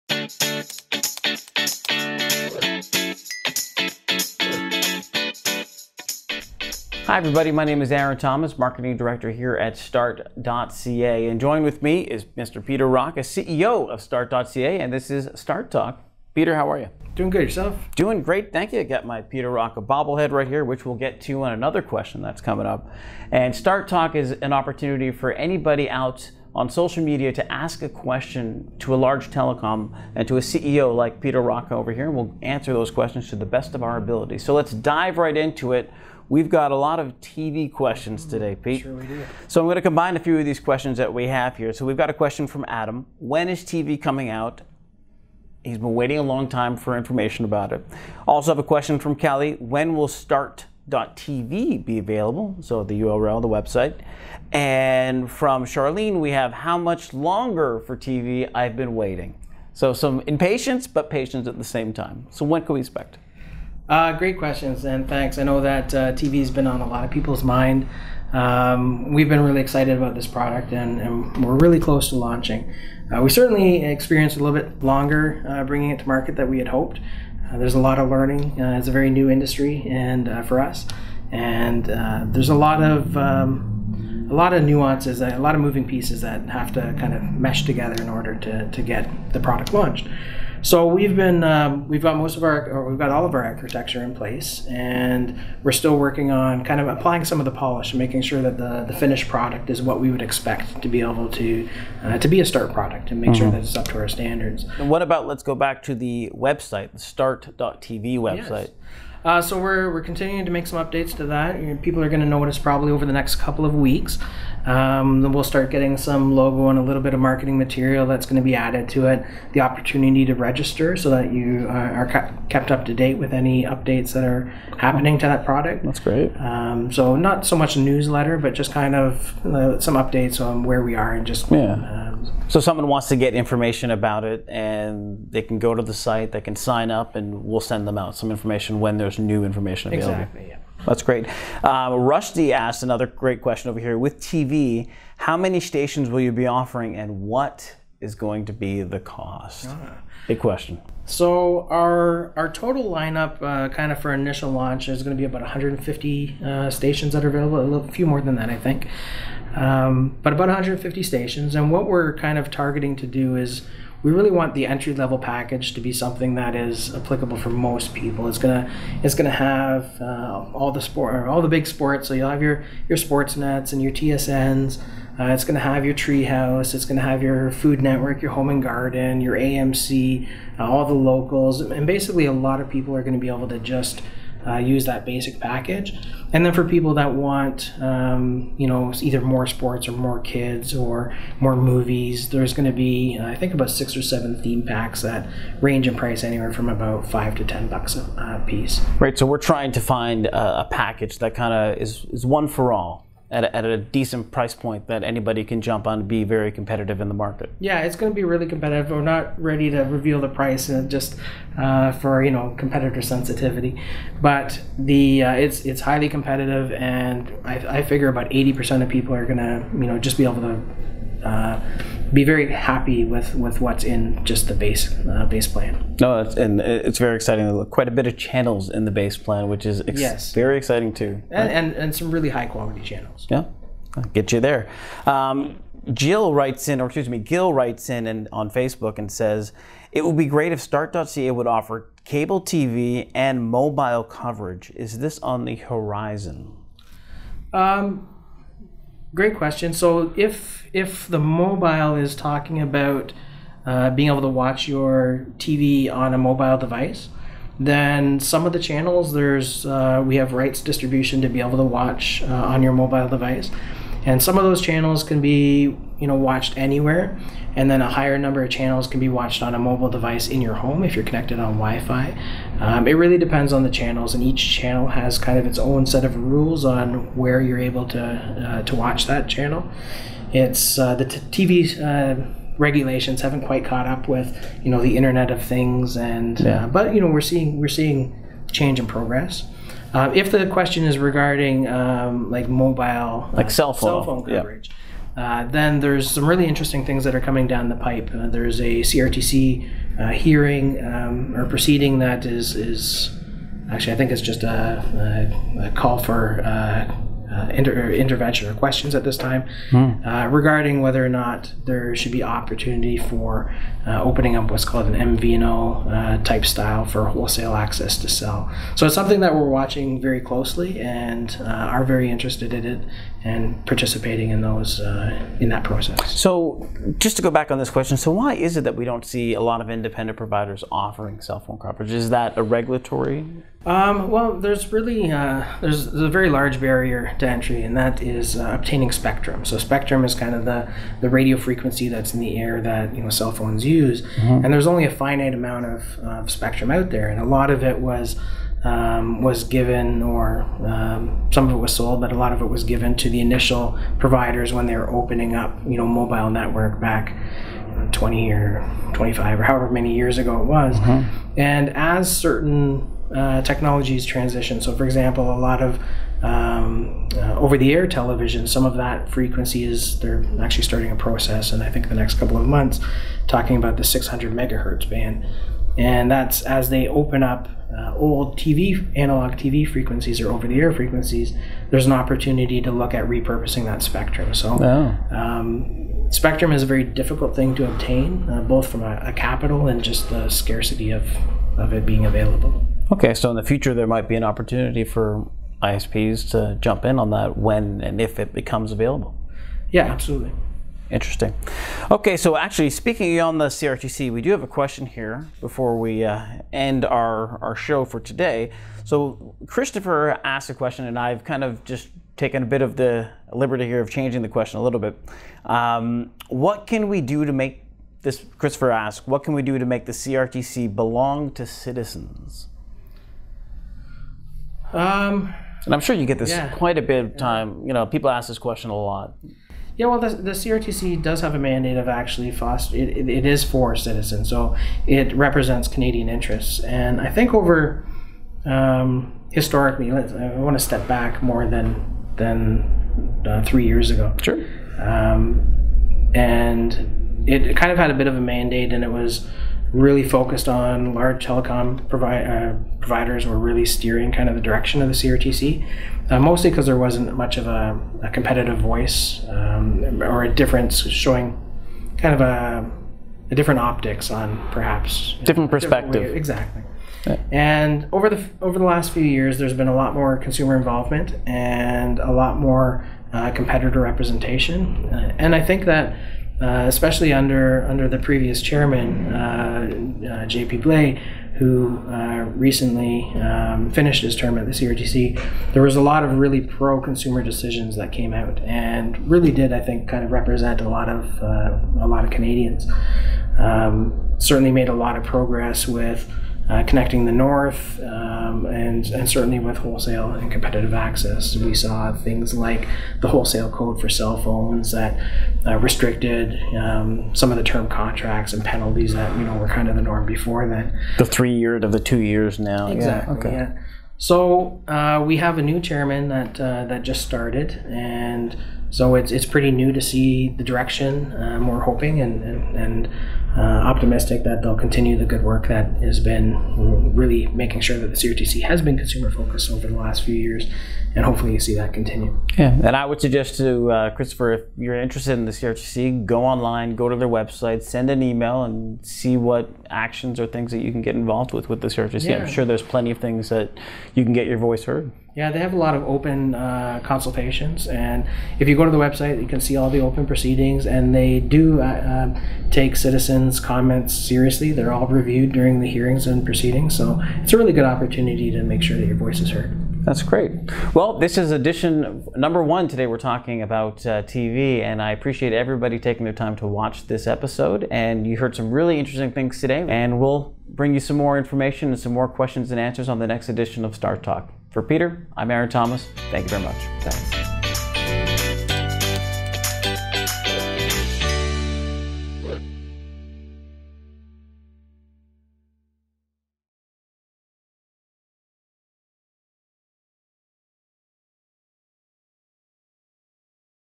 Start Talk Ep. 2 | Start.ca - CEO Peter Rocca answers your Start TV related questions
Enjoy another sit down with Start.ca CEO Peter Rocca, as he answers your all your Start TV related questions!

In episode two, Peter and our Marketing Director Aaron Thomas, go deep into topics like:

- When will Start TV launch?
- Start TV package lineups
- Cable TV and mobile coverage
- How to be more involved with the CRTC